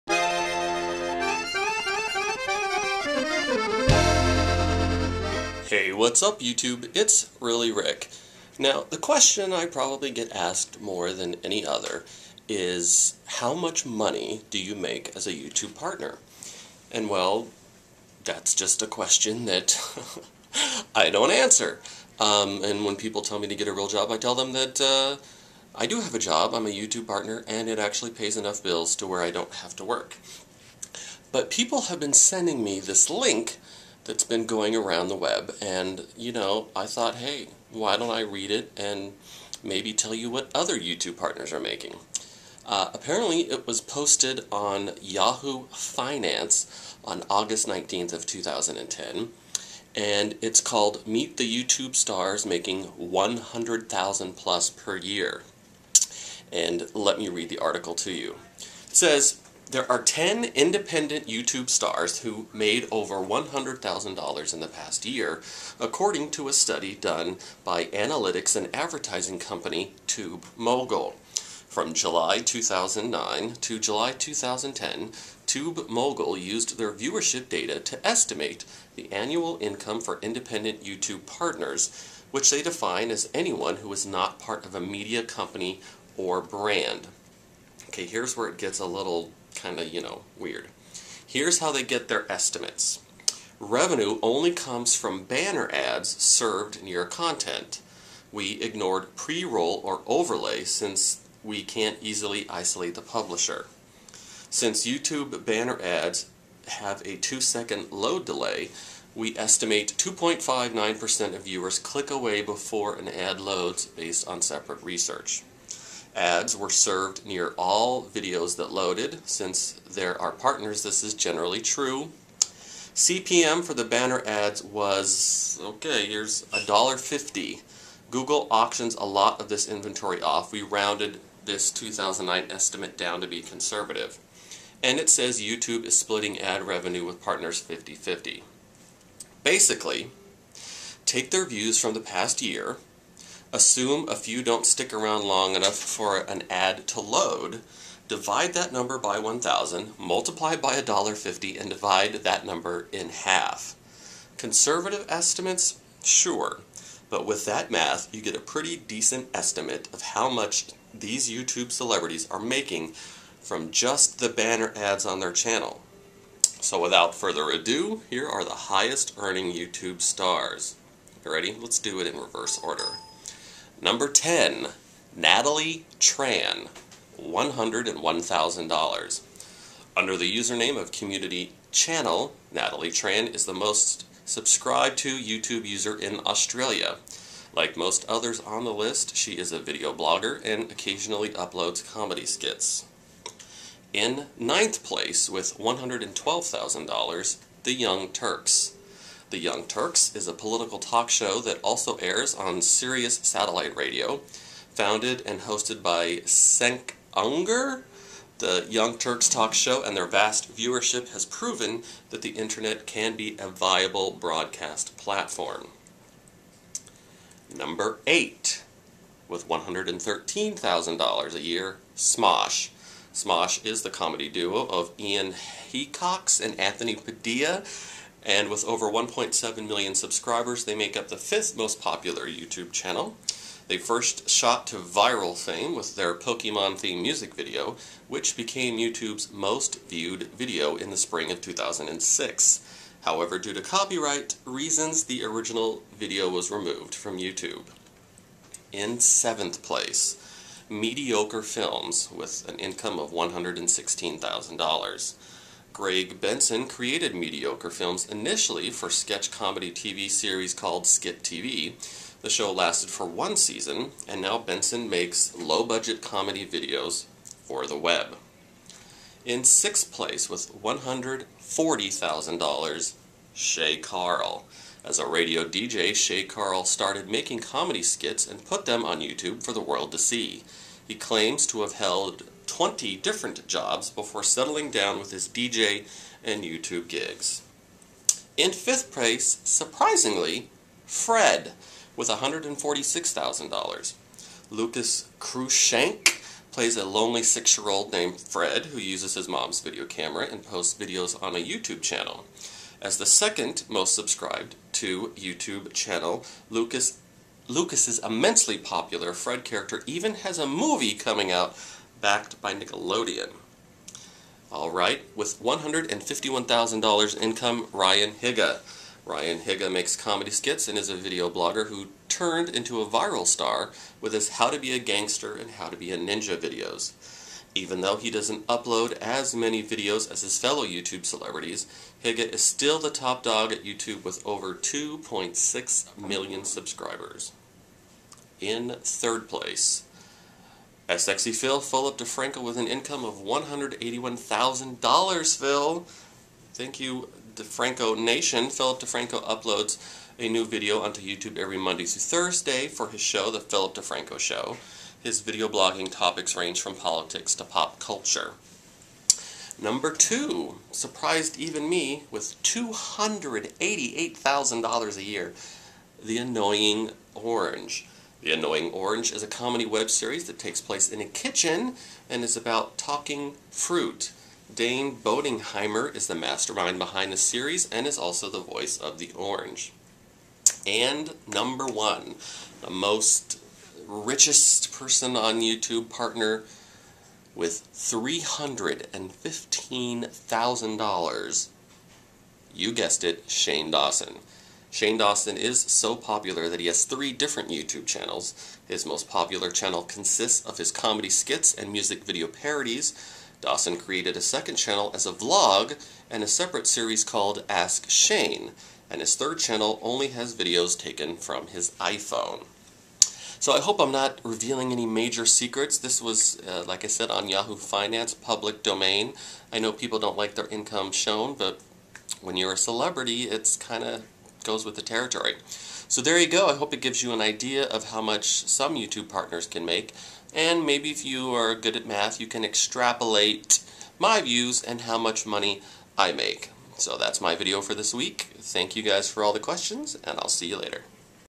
YouTube Partners - How Much $ Do They make? SEE HERE!
People constantly ask me how much money do I make as a YouTube partner.  Well a recent article on Yahoo Finance blows the lid of this closely guarded secret and gives an inside look at just how much cash money the Top YouTube "stars" maybe making.  After reading this story I for one went O.o.